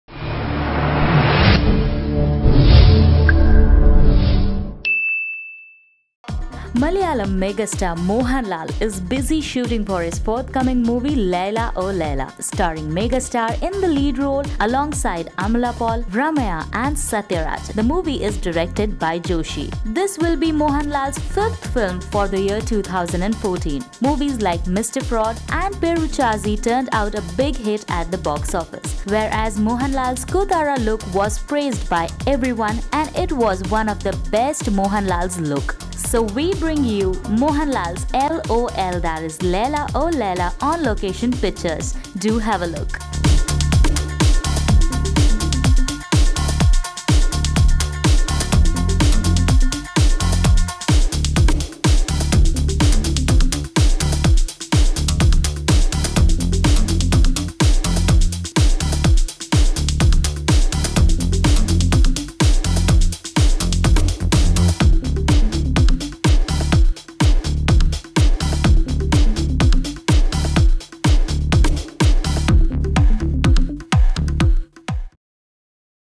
Laila O Laila On Location Pictures | Mohanlal | Amala Paul
Check out the unseen pictures of Mohanlal's upcoming movie 'laila o laila'. Movie is directed by Joshi, Amala Paul & Satyaraj also stars in the movie. Do have a look.